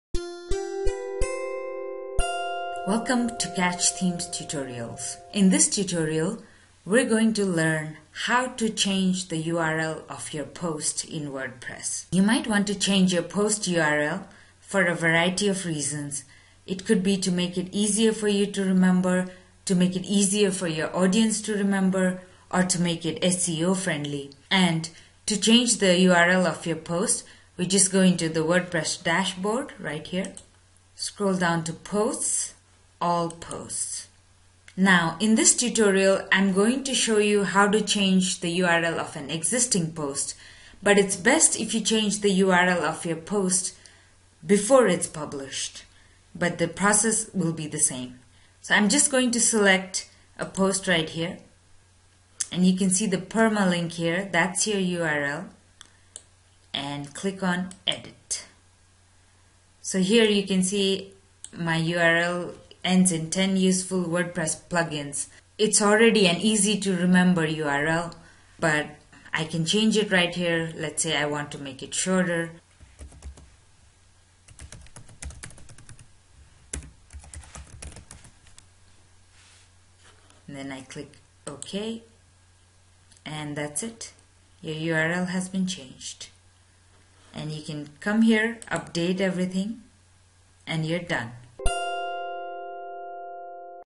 Tutorial: How to edit the url of your WordPress post.
Short tutorial on how you can edit the url of your WordPress post to make it easy to remember, or more SEO friendly.